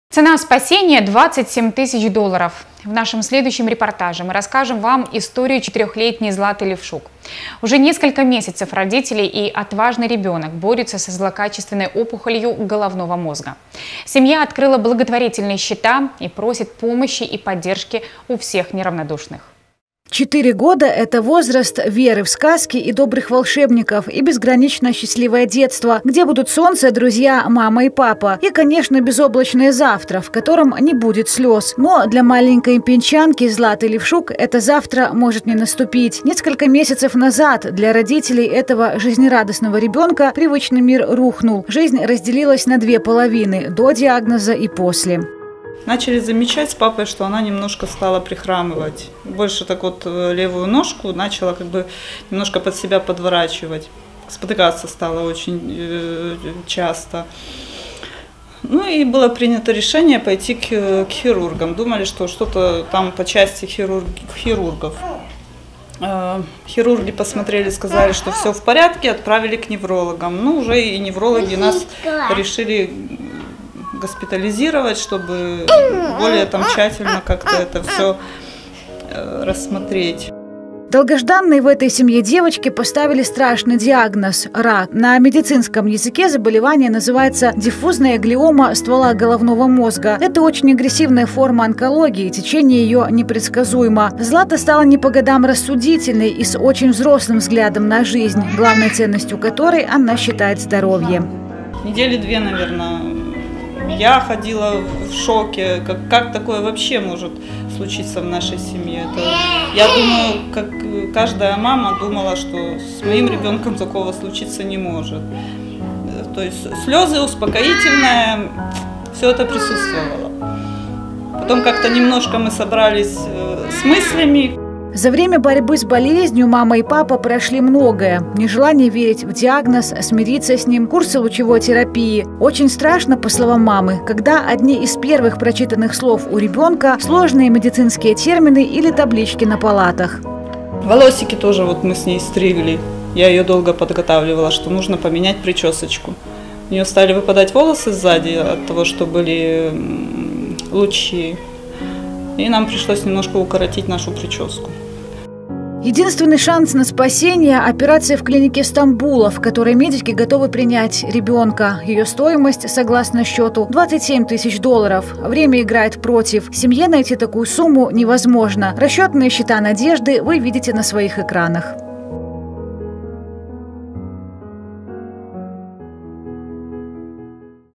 Не останьтесь равнодушными. Четырехлетняя Злата Левшук нуждается в помощи
Цена спасения – 27 тысяч долларов. В нашем следующем репортаже мы расскажем вам историю четырехлетней Златы Левшук. Уже несколько месяцев родители и отважный ребенок борются со злокачественной опухолью головного мозга. Семья открыла благотворительные счета и просит помощи и поддержки у всех неравнодушных.